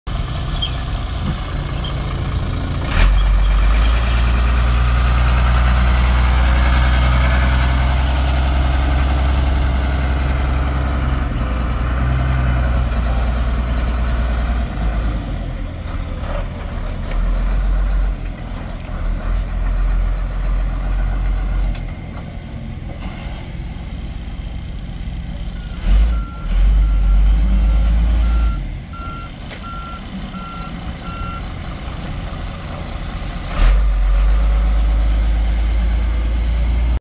1990 Ford F600 flatbed truck for sale at auction | bidding closes June 15, 2022
1990 Ford F600  flatbed truck
 Miles: 49,580 on odometer
 VIN: 1FDMK64P5LVA45735
Engine
 Cummins
 Displacement: 6.6L
 Cylinders: 6
 Fuel type: Diesel
Transmission
 Automatic
Chassis
 Axles: Single
 Suspension: Spring
 Brakes: Hydraulic
 GVWR: 19,000 lbs
 Wheelbase: 171"
Interior
 Heat
Features
 Flatbed
 15' L x 96" W
 Removable sides
 End gate lift
 Venco
 Capacity: 2,000 lbs
Tires
 Front: 11R22.5
 Rear: 10R22.5
Notes
 Bed contents not included
 Unknown operating condition of end gate lift
Missouri title
It's located in Saint Peters, MO, and we're selling it to the highest bidder regardless of price.